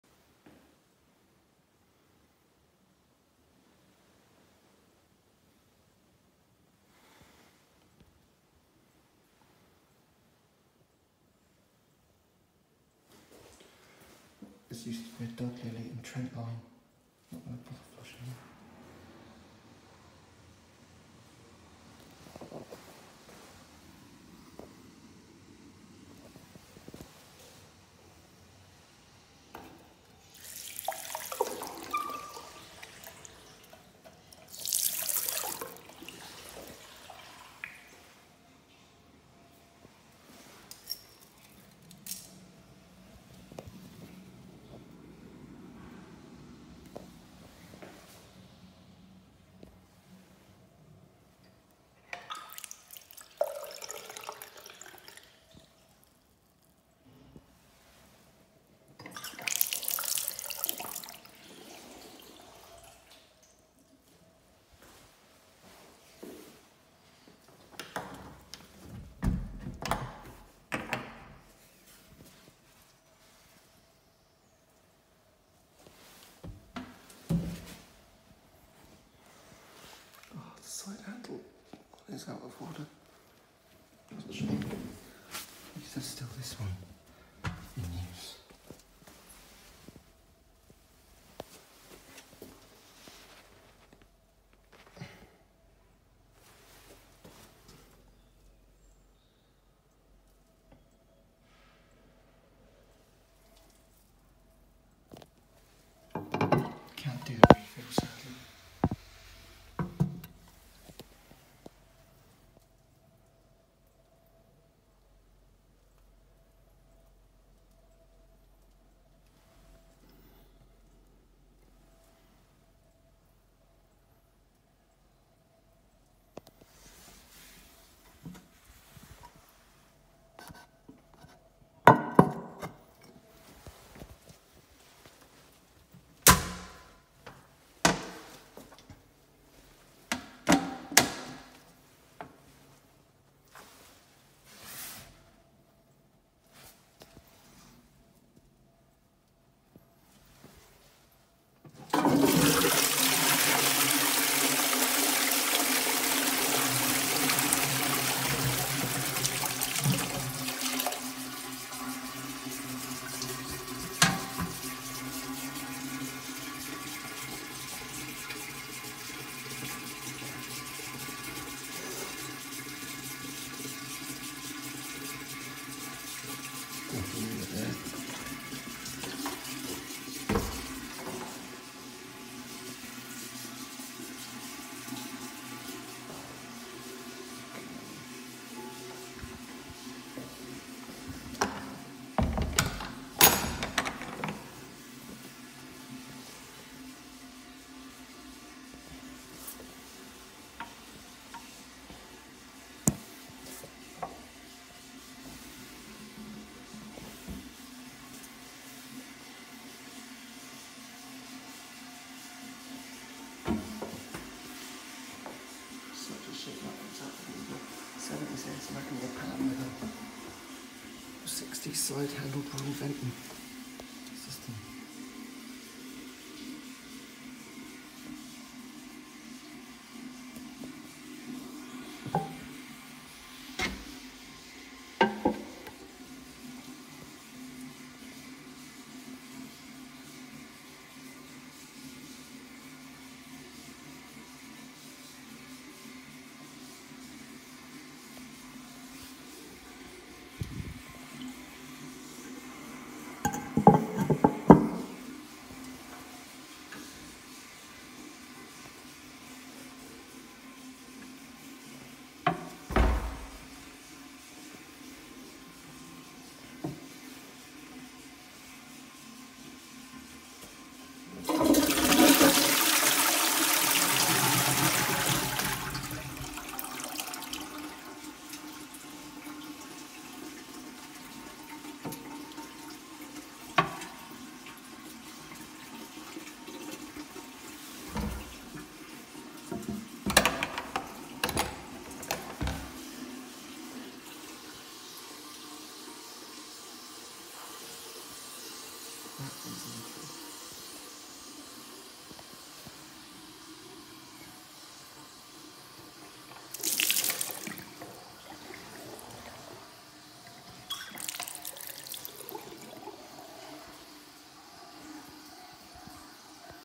LL 1980’s AS Kensington cistern and + LL 1960’s Armitage Ware Kensington cistern
L’s eq
Pans are a 2000’s AS magnia and a 80’s Twyfords Classic.

And a few changes have taken place here too. First of all the 2000’s Dudley Elite and 90’s Trent Trentline pan have been replaced in favour of a shoddy CCPB and the middle cubicle is out of order. It’s a 70’s AS magnia pan with a 1960’s side handle Royal Venton Coronet cistern. Not sure what’s wrong with it and would have tried to troubleshoot it had the door not been screwed shut. I just hope to god it doesn’t end up like the 4th cubicle.